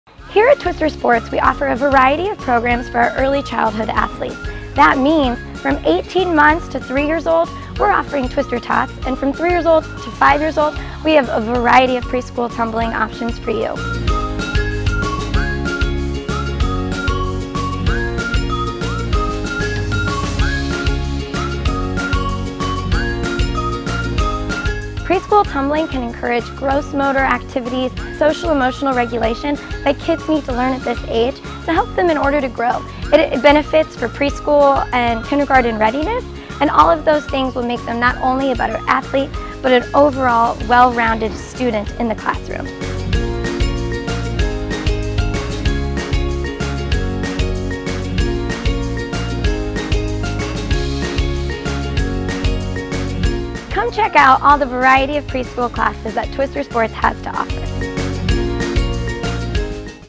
Twister Sports Preschool Programs
Twister Sports offers a variety of recreational programming for preschool-aged students. Our coaches are excellent at helping focus your 2-5 year old and teaching them fun skills and preparing them for kindergarten! Check out the great programs we have at twistersports.com:

Preschool Tumbling, Preschool Combo Tap & Ballet, Wiggle Worms, Twister Tots, Ninja Tots, Mini Ninjas, Birthday Parties, Daytime Playtime

Twister Sports
240 NW 13 Hwy
Warrensburg, MO 64093